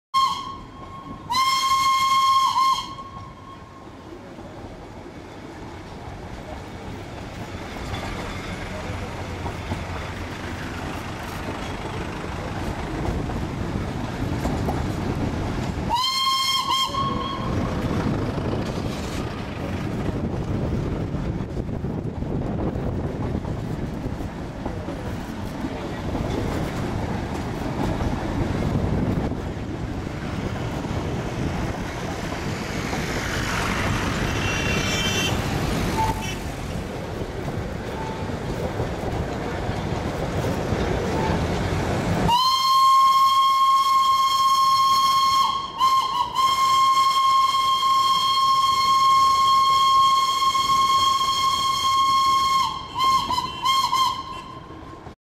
Dali Gonpa 03
Dali Gonpaと呼ばれるチベット寺院Drug Sangag Choelingの前を通る9D列車